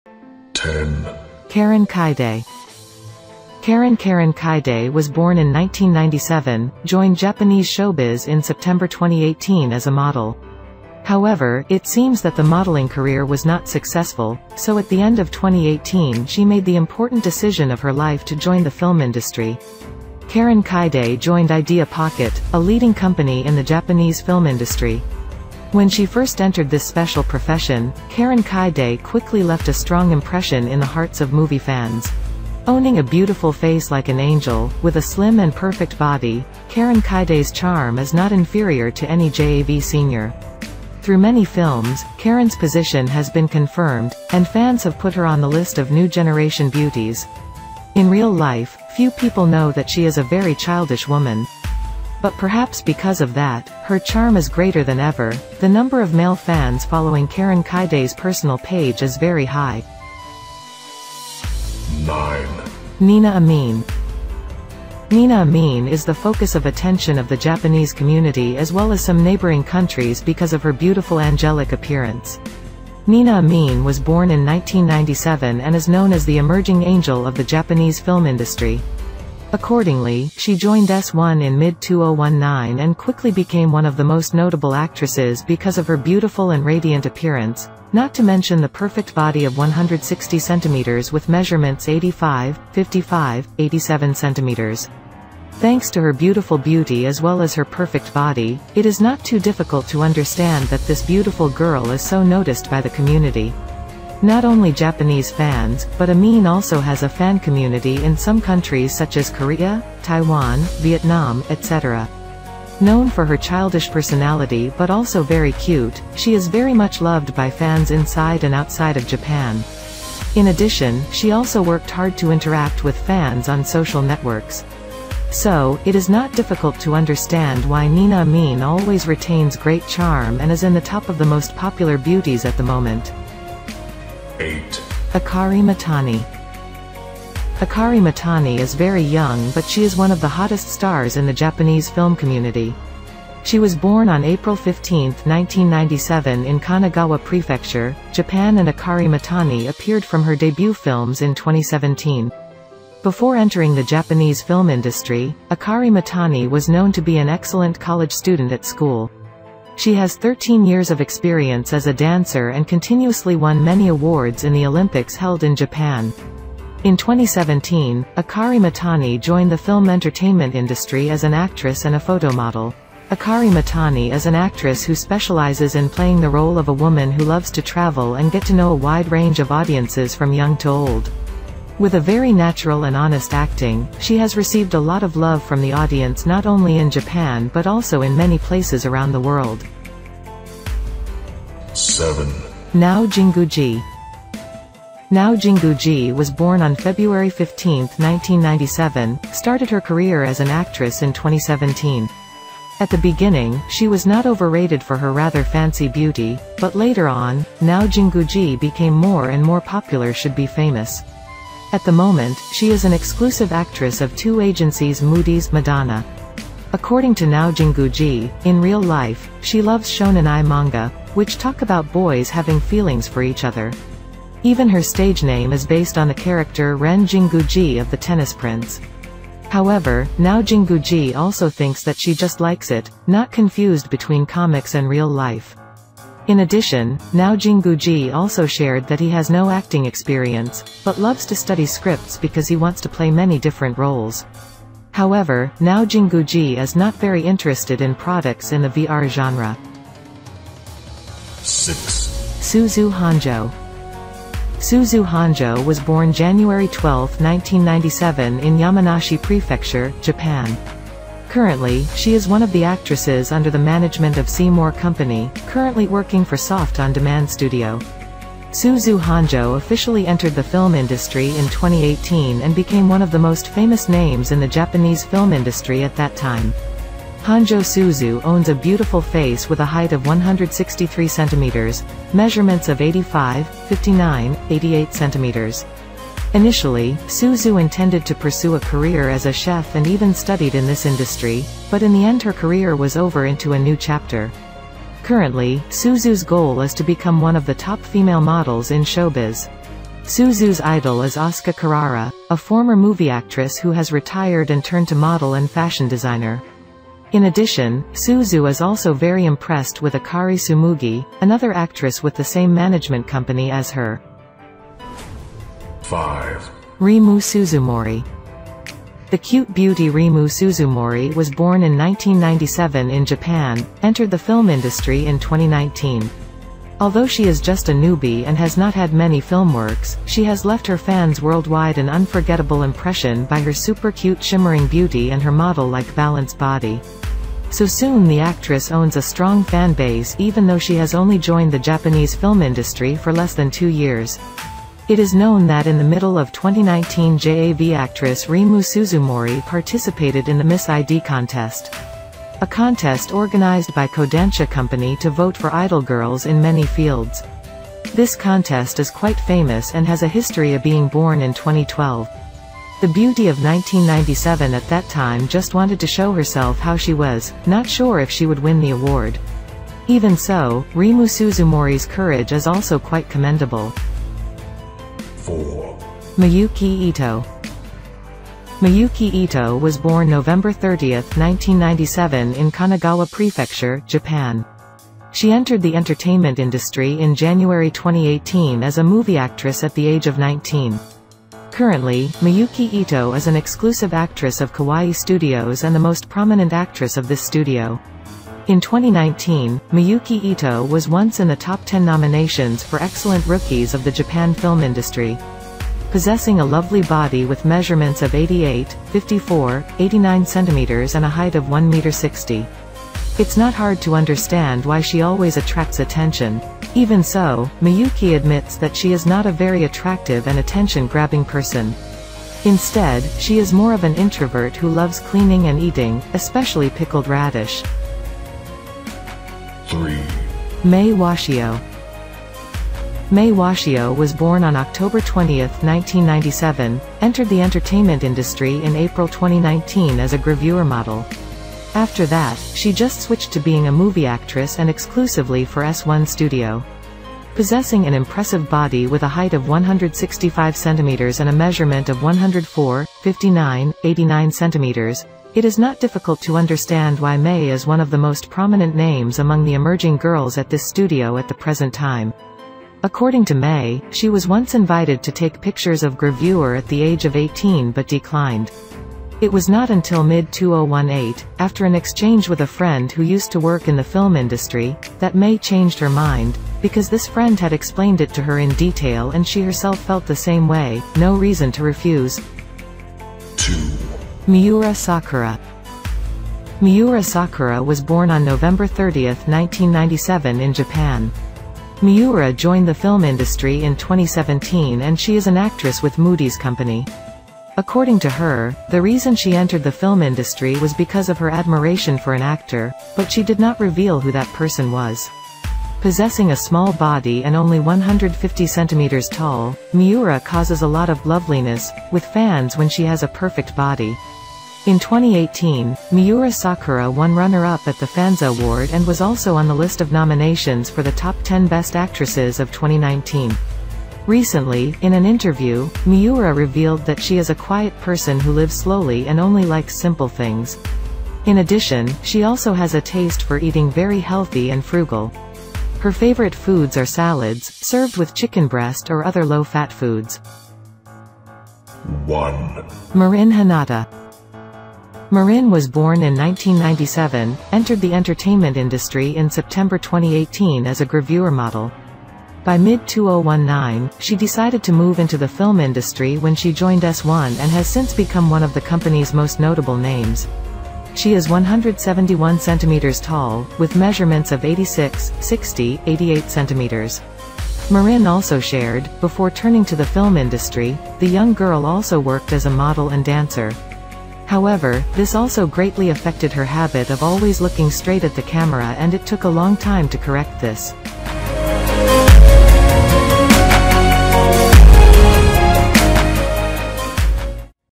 TOP 10 best JAV girls born in 1997
1. Marin Hinata
Marin was born in 1997, entered the entertainment industry in September 2018 as a gravure model. By mid-2019, she decided to move into the film industry when she joined S1 and has since become one of the company's most notable names.

She is 171 cm tall, with measurements of 86, 60, 88 cm. Marin also shared, before turning to the film industry, the young girl also worked as a model and dancer. However, this also greatly affected her habit of always looking straight at the camera and it took a long time to correct this.
2. Miura Sakura
Miura Sakura was born on November 30, 1997 in Japan. Miura joined the film industry in 2017 and she is an actress with Moodyz company. According to her, the reason she entered the film industry was because of her admiration for an actor, but she did not reveal who that person was.

Possessing a small body and only 150 cm tall, Miura causes a lot of "loveliness" with fans when she has a perfect body. In 2018, Miura Sakura won runner-up at the Fanza Award and was also on the list of nominations for the top 10 best actresses of 2019.

Recently, in an interview, Miura revealed that she is a quiet person who lives slowly and only likes simple things. In addition, She also has a taste for eating very healthy and frugal. Her favorite foods are salads, served with chicken breast or other low-fat foods.
3. Mei Washio / Kakei Jun
4. Mayuki Ito
5. Remu Suzumori
6. Honjo Suzu
7. Nao Jinguji
8. Akari Mitani
9. Nina Amin
10-Karen Kaede